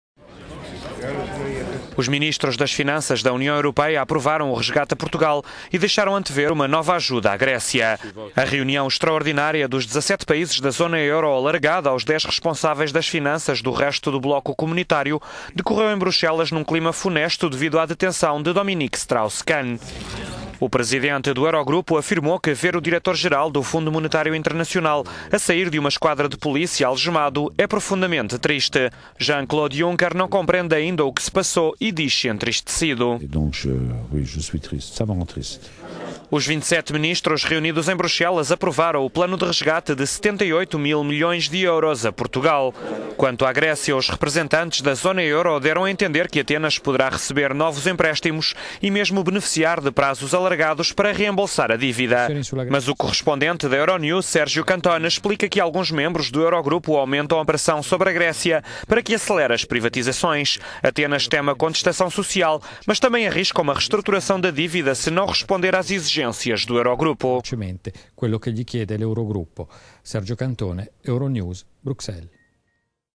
UE aprova resgate a Portugal e Zona Euro admite nova...
Os ministros das Finanças da União Europeia aprovaram o resgate a Portugal e deixaram antever uma nova ajuda à Grécia.

A reunião extraordinária dos 17 países da Zona Euro, alargada aos dez responsáveis das Finanças do resto do bloco comunitário, decorreu em Bruxelas num clima funesto devido à detenção de Dominique Strauss-Kahn.